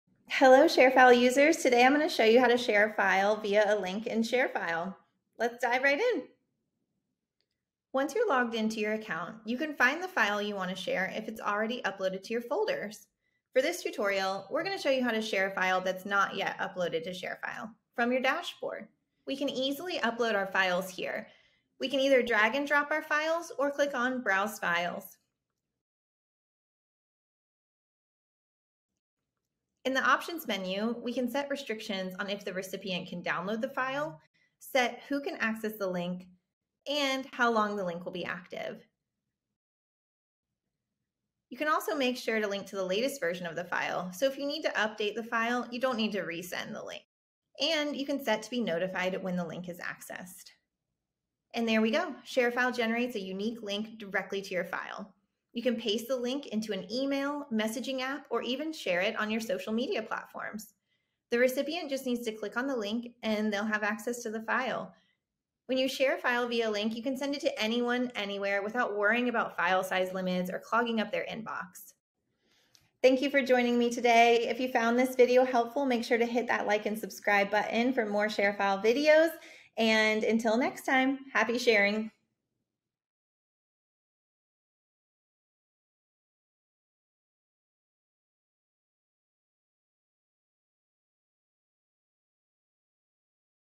How to Share a File Using a Link
This video will show you how to share a file via a link in ShareFile — after all we have a company name to live up to!